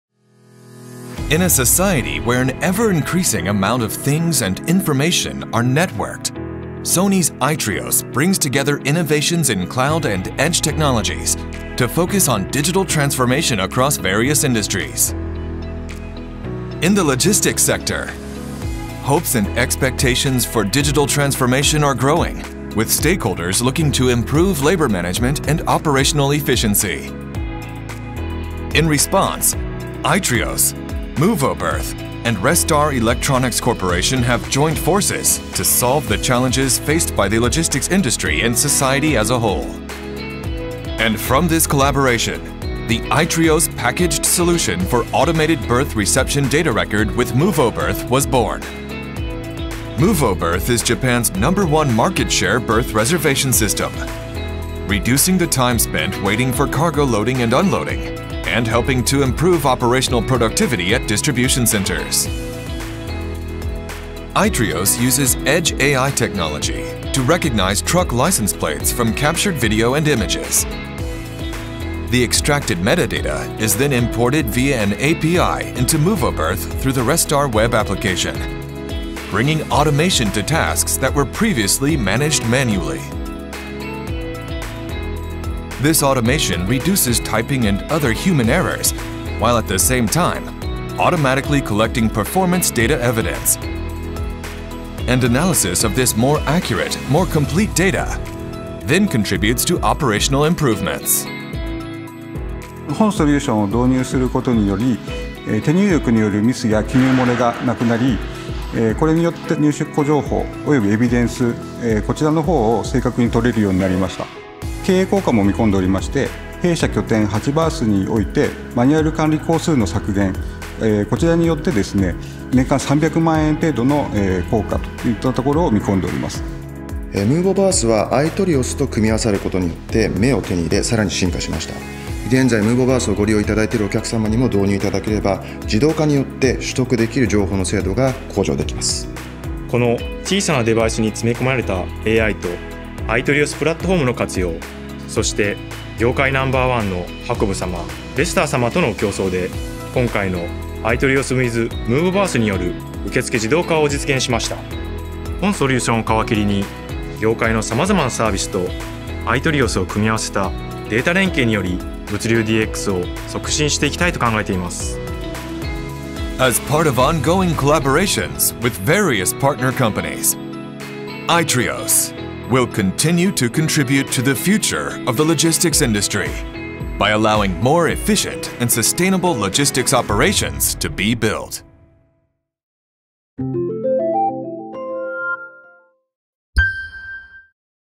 AITRIOS|Packagedsolution for automated berth reception data record with MOVO Berth | Sony Official
A new solution to improve operations in the logistics industry was born, utilizing Sony's edge AI sensing platform, AITRIOS™.
AITRIOS will continue to contribute to the improvement of productivity in the logistics industry through DX.
For more information about AITRIOS, please visit: AITRIOS | Sony Semiconductor Solutions Group